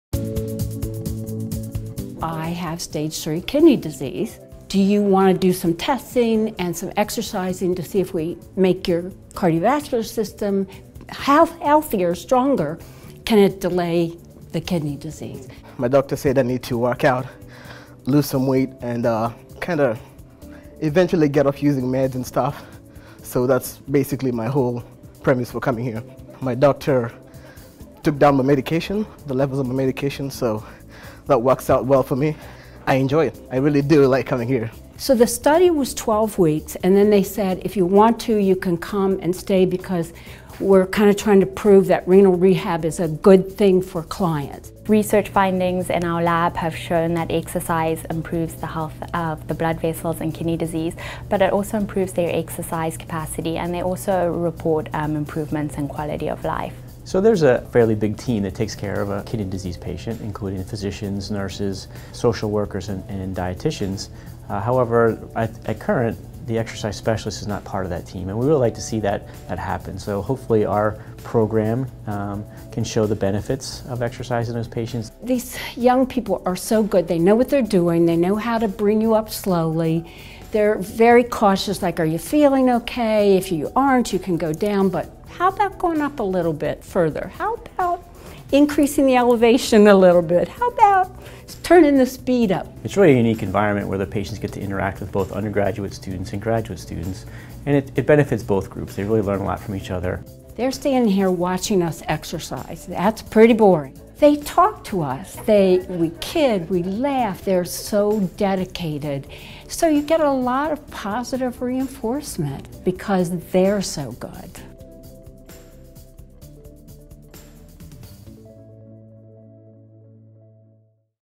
Renal rehab at UD combats chronic kidney disease
A University of Delaware research team in the College of Health Sciences is combating chronic kidney disease (CKD) with exercise. Renal rehab study results showed an exercise program improved blood vessel health and exercise capacity. Equally as important, patients reported improvements in their everyday quality of life as a result of becoming more active.